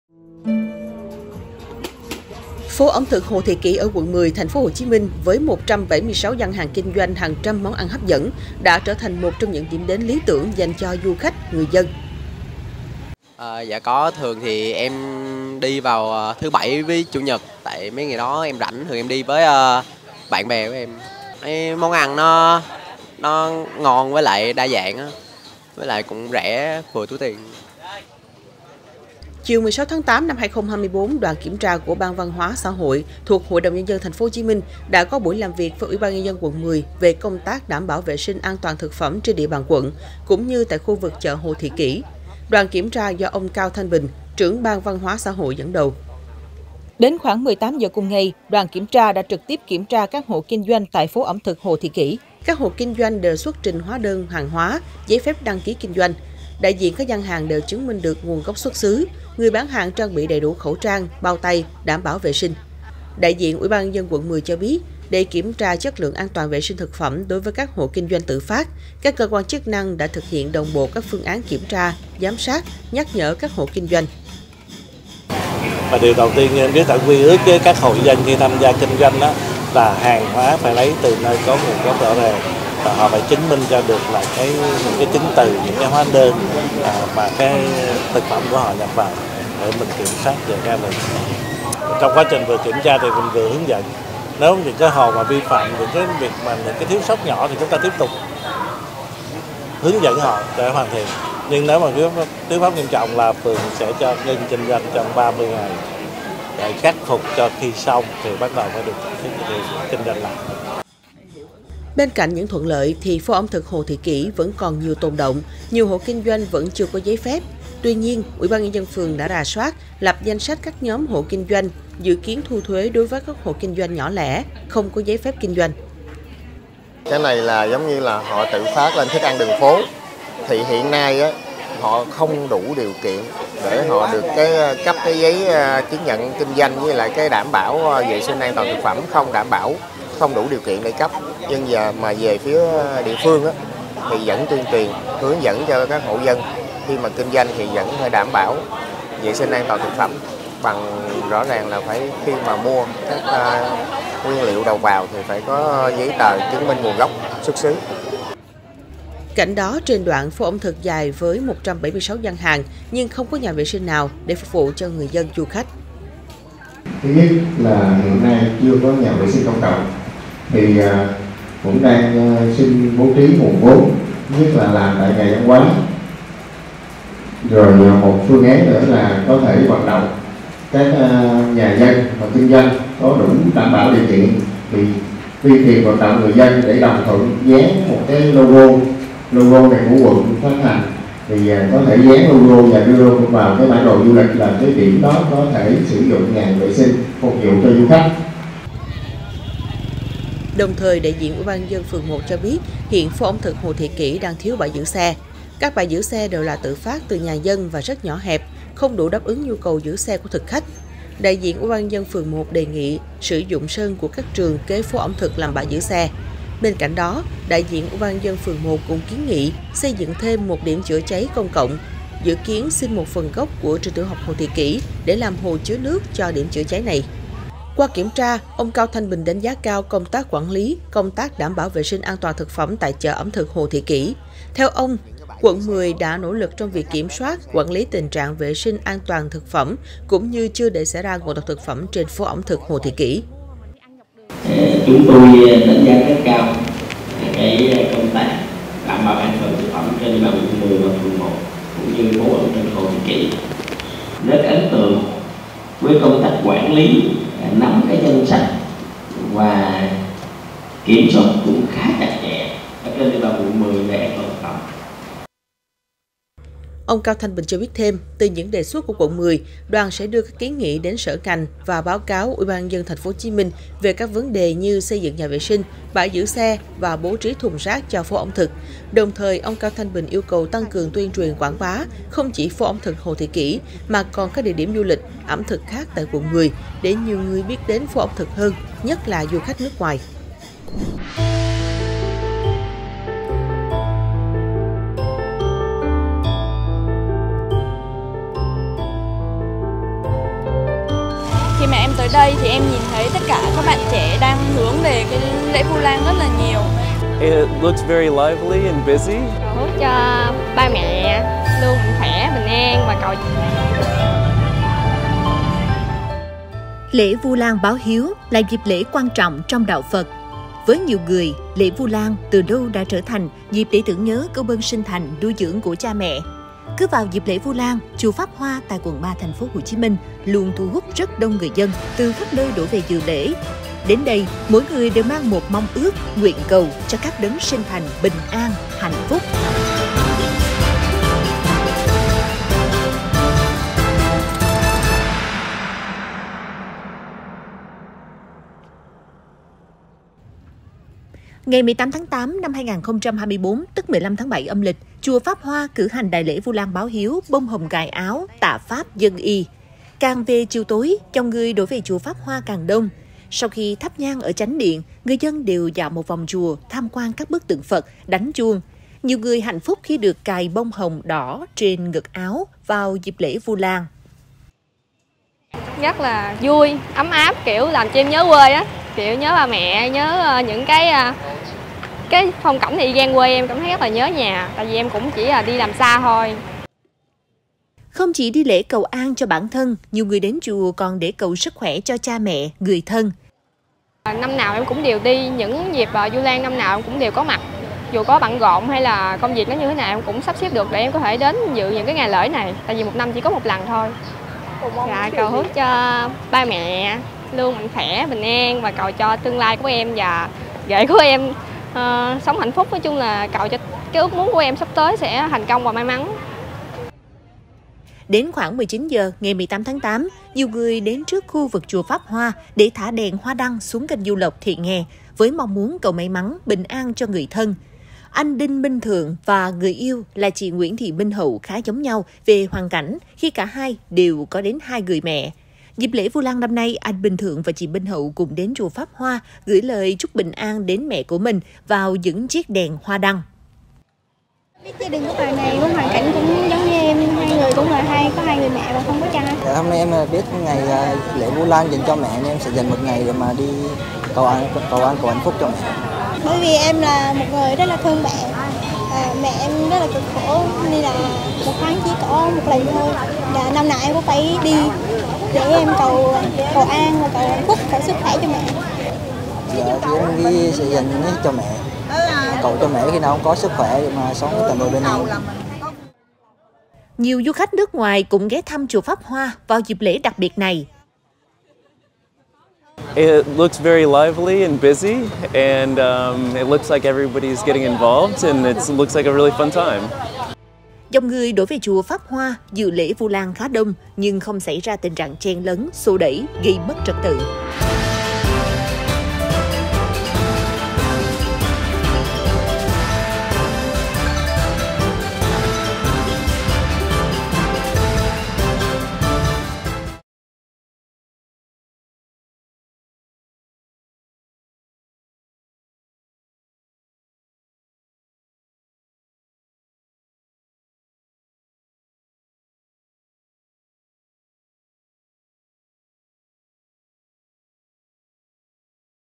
Tháo gỡ tồn đọng để phát triển phố ẩm thực Hồ Thị Kỷ
0:00 Tháo gỡ tồn đọng để phát triển phố ẩm thực Hồ Thị Kỷ
4:50 Ước mong ngày Vu lan: ‘Cầu cho mẹ có sức khỏe để khi nào cũng sống bên em'

Ban Quản lý phố ẩm thực Hồ Thị Kỷ (quận 10, TP.HCM) đề xuất xây dựng thêm nhà vệ sinh, bãi giữ xe, thùng rác… để phục vụ nhu cầu vui chơi, ăn uống của người dân, du khách.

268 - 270 Nguyễn Đình Chiểu, Phường Võ Thị Sáu, Q.3, TP HCM